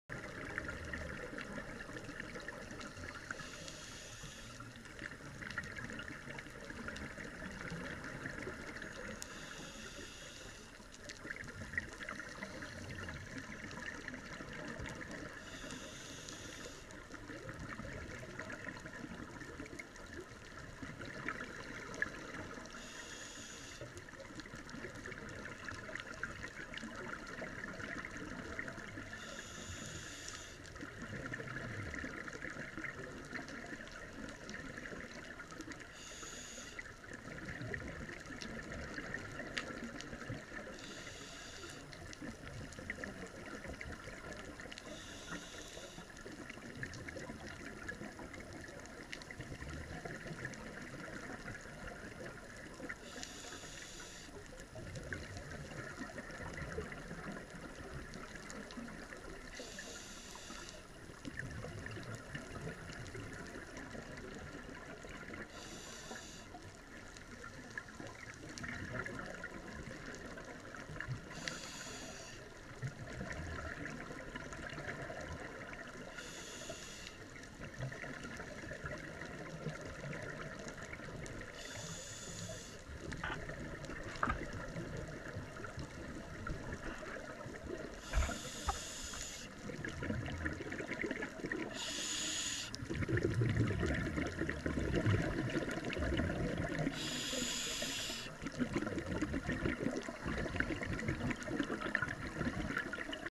Anchor off the starboard bow of The Benwood, 3/1/18
This anchor was uncovered by Hurricane Irma.  It was buried in the sand for many years, but is now visible a short swim off of the starboard bow of Wreck of The Benwood.  Look for the ship's bollards and then go beyond them.